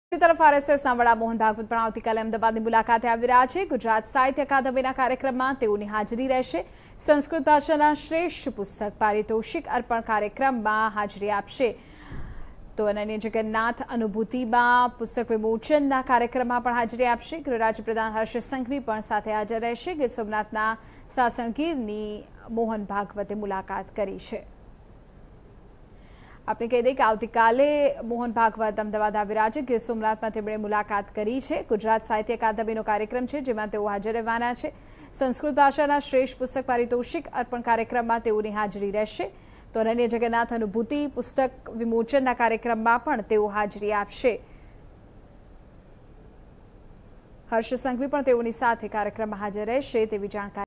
RSS chief Mohan Bhagwat to arrive Ahmedabad tomorrow to attend Gujarat literature academy event| TV9
RSS chief Mohan Bhagwat to arrive Ahmedabad tomorrow to attend Gujarat literature academy event| N45